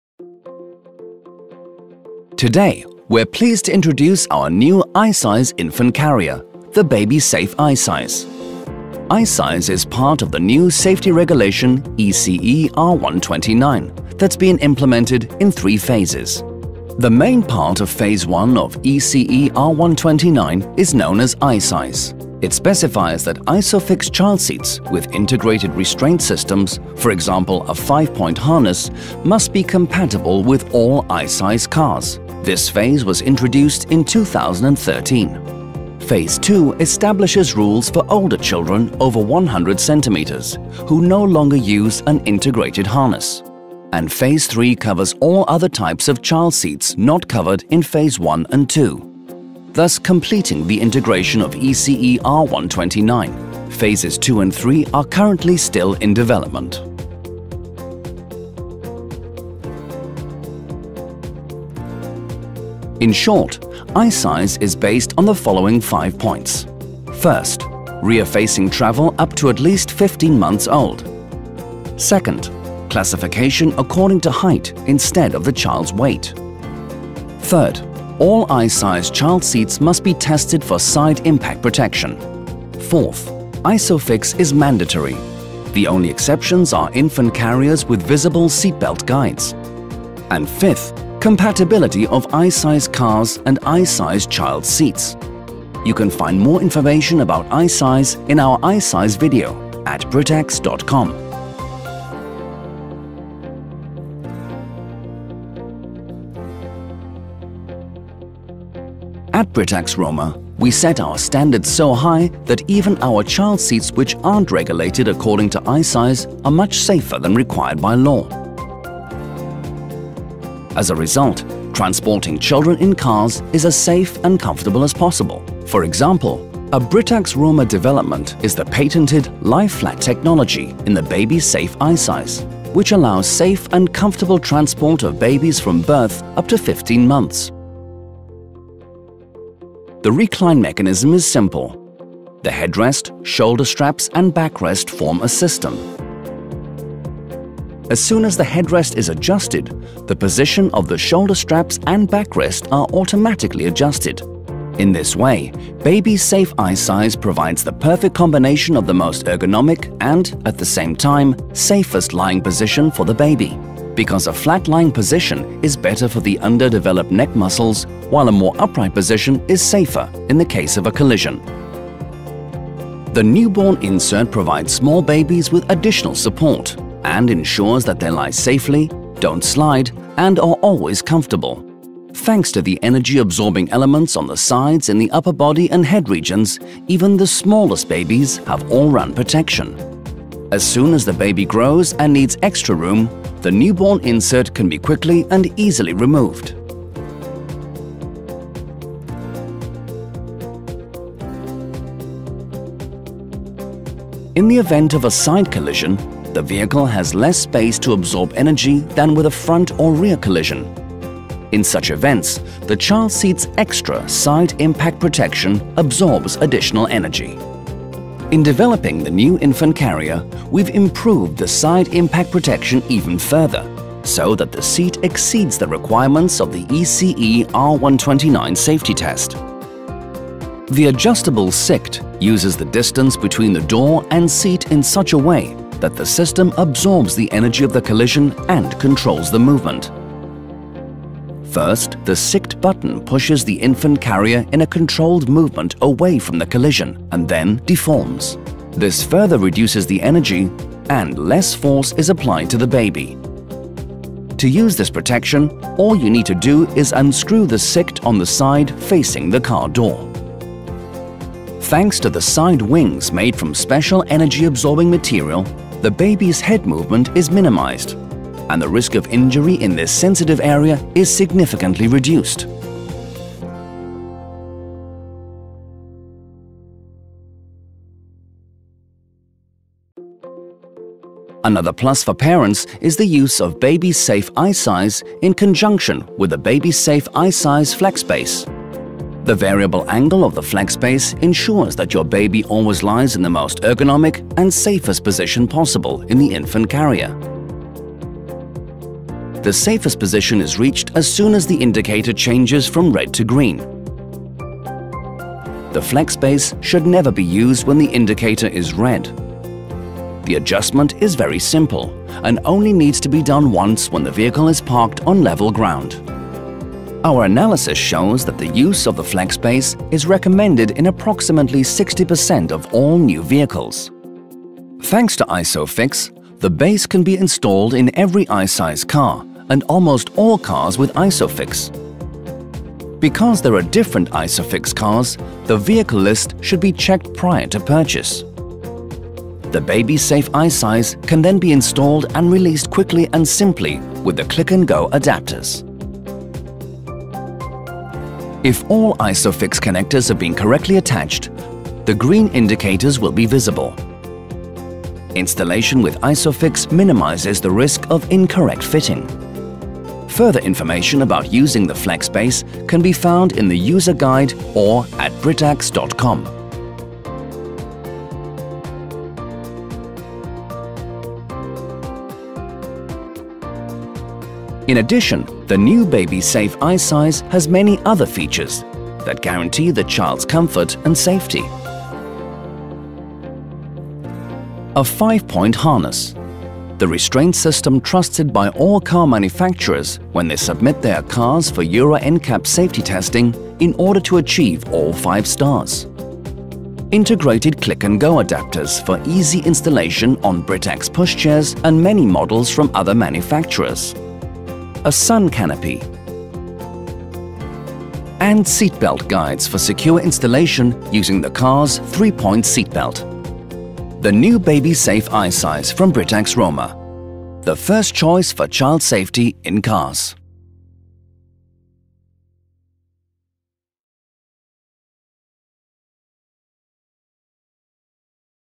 Видеоинструкция автокресла BABY SAFE i SIZE
Автокресло Baby-Safe2 i-size разработано в соответствии с новым стандартом безопасности детских автомобильных кресел ECE R129 (i-size). Автокресло оснащено запатентованным встроенным механизмом lie-flat, благодаря которому высота подголовника регулируется одновременно с углом положения ребёнка в люльке.